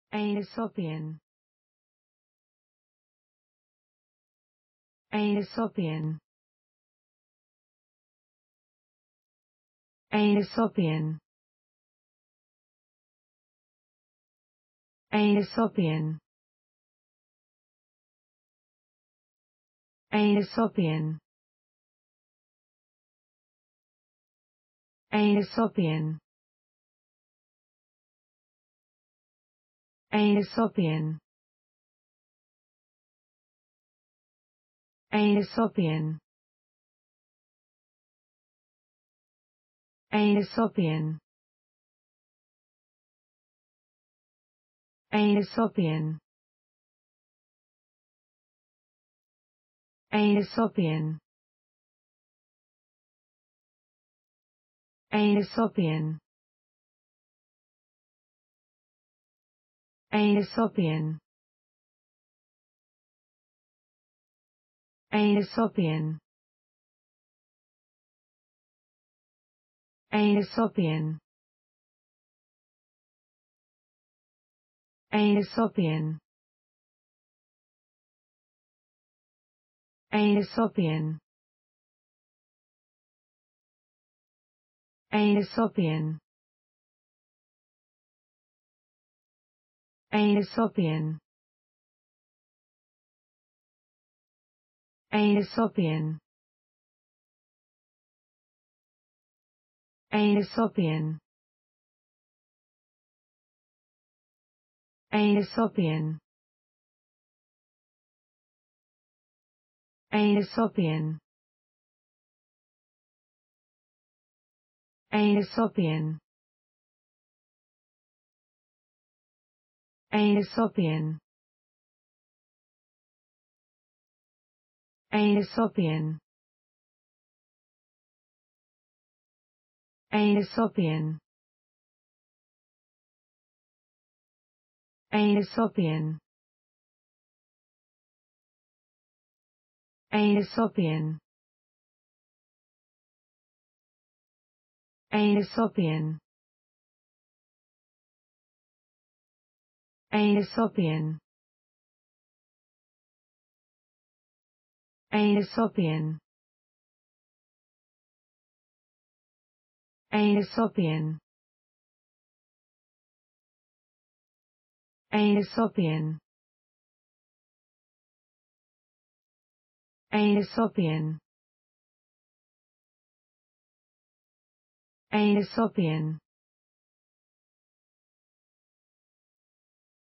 how to pronounce the english word Aesopian.amazing resource.learn from me.
Follow me!Learn from me how to pronounce the english word Aesopian.This video shows you how to pronounce the English word  Aesopian .  the word means   伊索的 or 伊索的 or yi suo de.